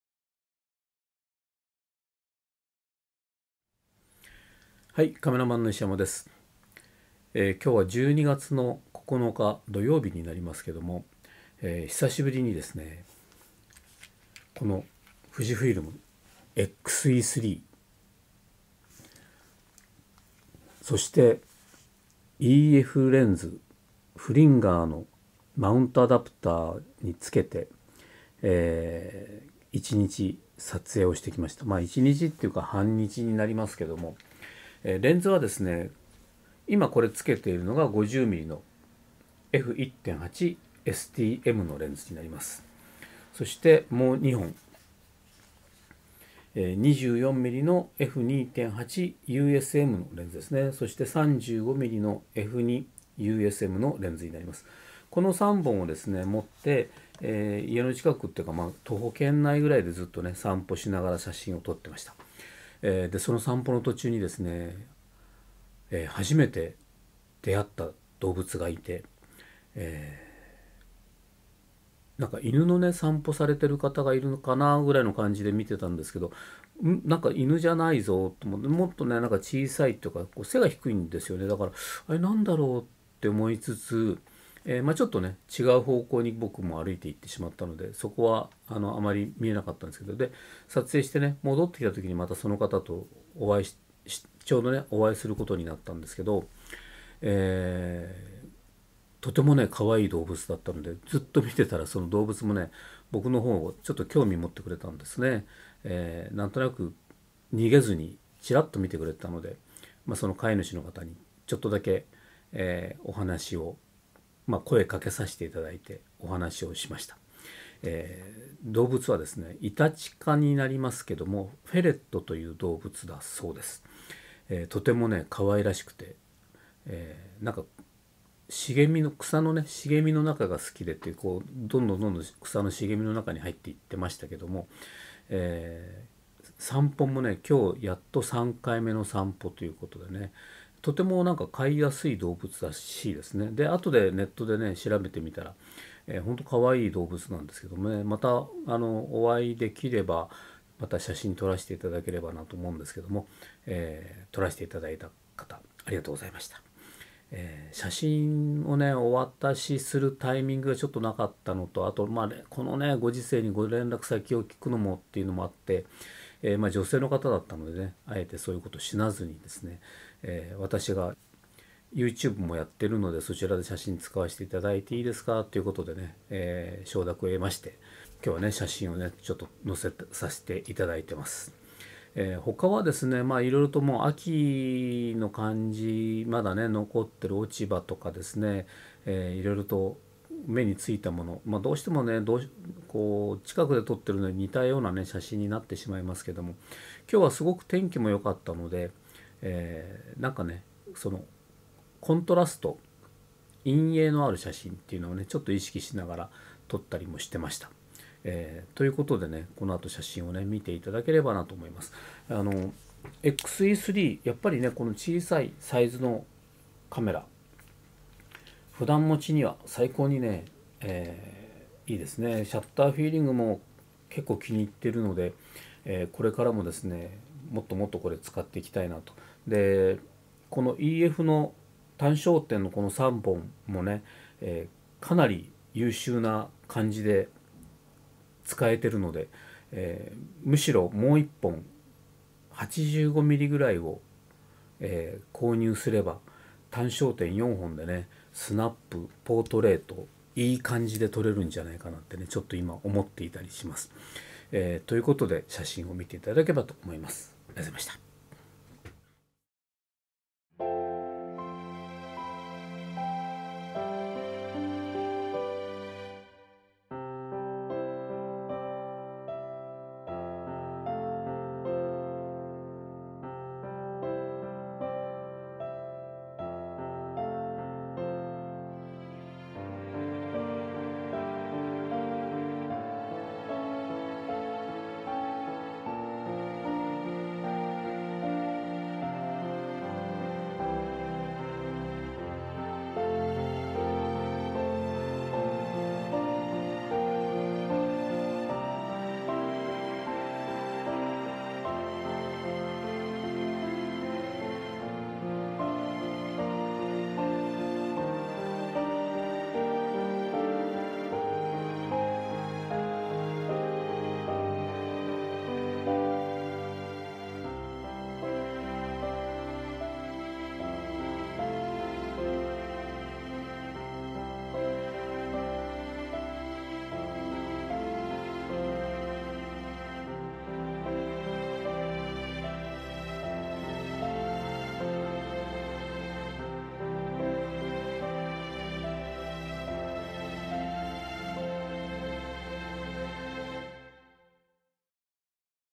FUJIFILM X-E3 ➡️ fringer ➡️ Canon EFレンズで撮影しました。
FUJIFILM X-E3にfringerマウントアダプターを使用してEFレンズで撮影しました。EF50mm EF35mm EF24mm との相性も良く、サクサクと撮影出来ました。